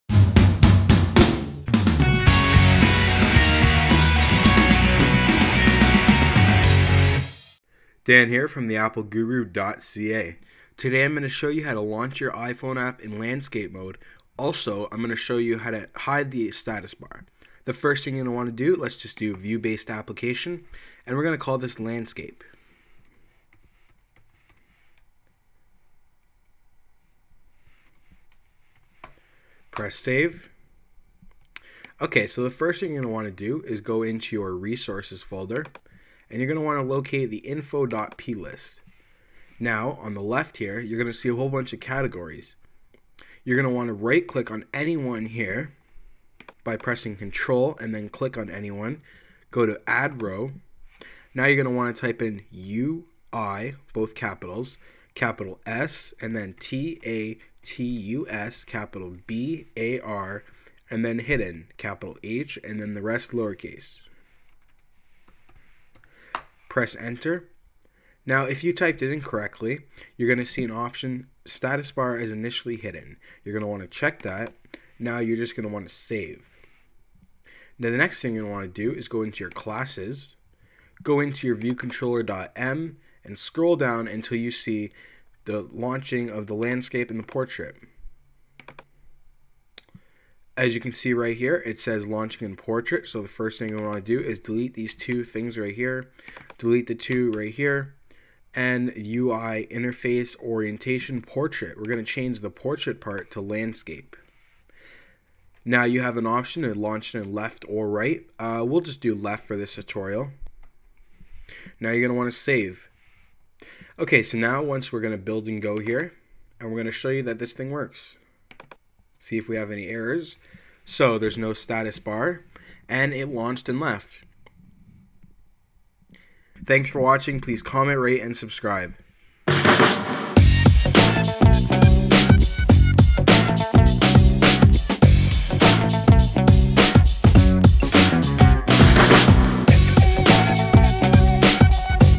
iPhone SDK - How To Launch iPhone App In Landscape
This is How To Launch an iPhone App In Landscape.
It WIll Also Show You How To Hide the status bar.